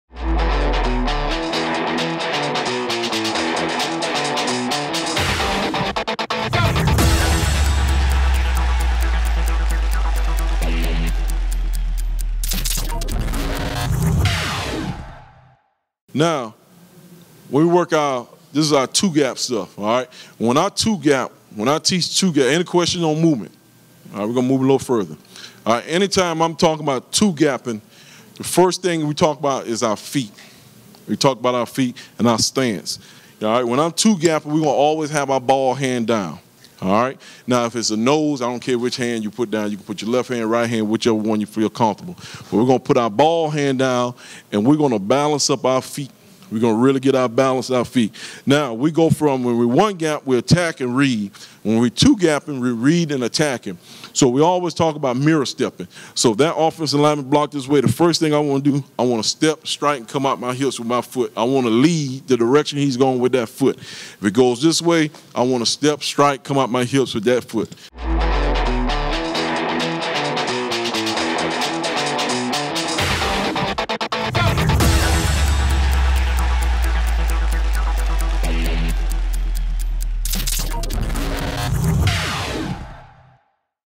Chris Rumph - Two Gap Technique
Chris Rumph, the Defensive Line Coach at the University of Florida, illustrates the two gap technique including the players stance and the importance of reading the opposing player and then attacking versus the one gap technique where you attack then read.